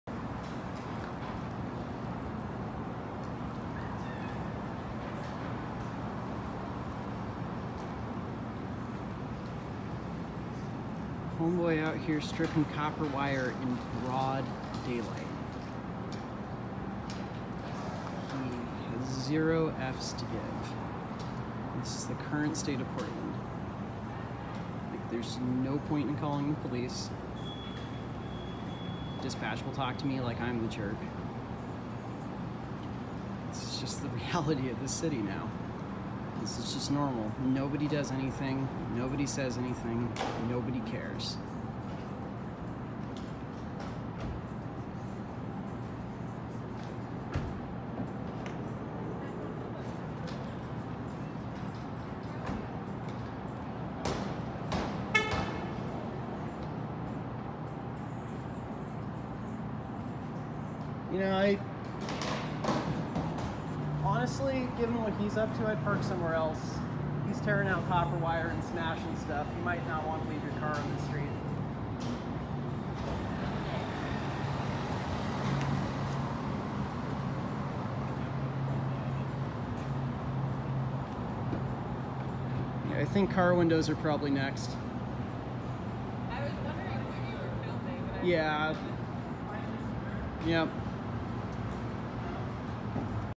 Knife Wielding Tweeker Steals Copper Wire From Sign (Portland Oregon)
Portland is beyond parody, something like this happens every time I go into downtown.